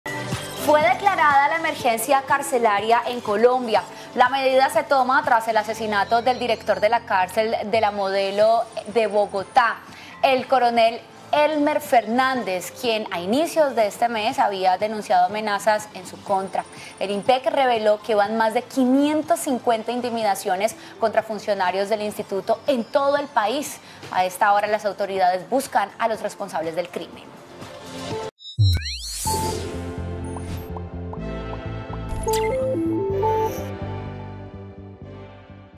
Declaran emergencia carcelaria tras asesinato del director de la cárcel La Modelo |17.05.2024| TPN
Colombia | La medida se toma tras el asesinato del director de la cárcel La Modelo de Bogotá, el coronel Élmer Fernández, quien a inicios de este mes había denunciado amenazas en su contra. El INPEC reveló que van más de 550 intimidaciones contra funcionarios del instituto en todo el país.